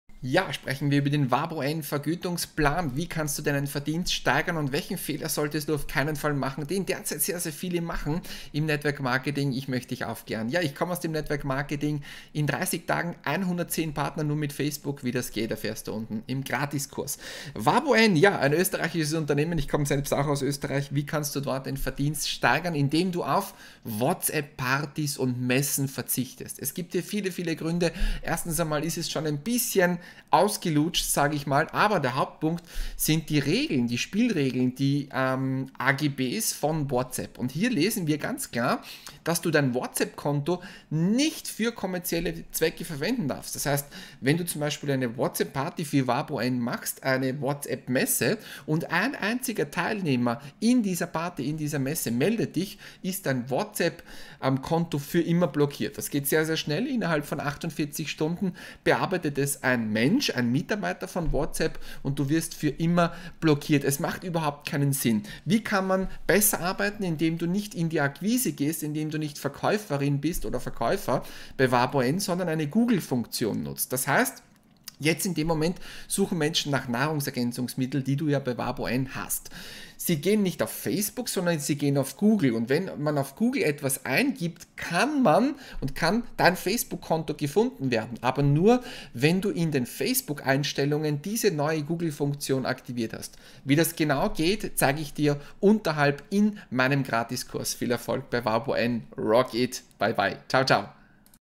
Vabon N Vergütungsplan: Der größte Fehler = wenig Verdienst!
Wie kannst du im Marketingplan - Verdienstplan als Vabo N Partner nach oben kommen? Wie kannst du mehr Geld verdienen und die Provision (Auszahlung) erhöhen als Vertriebspartner? Wie machst du den Vertrieb nur mit Facebook?